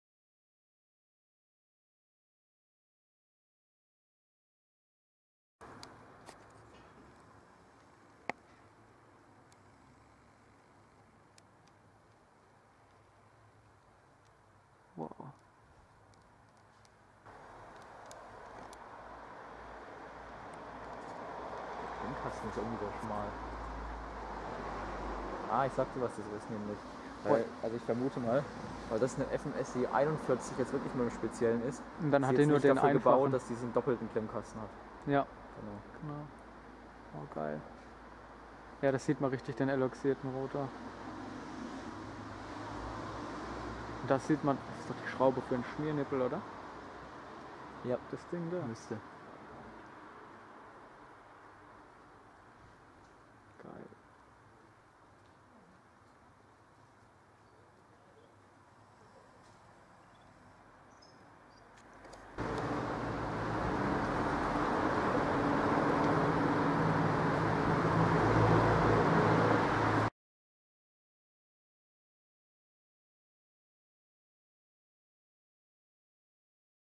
Luftschutzsirene Fmsi 41 von Siemens Schuckert
Bevor die nächste Folge vom Sirenenhunter mit einem neuen Abbau kommt, zeige ich euch hier eine schöne Fmsi 41 Luftschutzsirene. Die Sirene ist nicht mehr in Betrieb. Der eventuelle Abbau und somit die Rettung der Sirene steht noch in Klärung.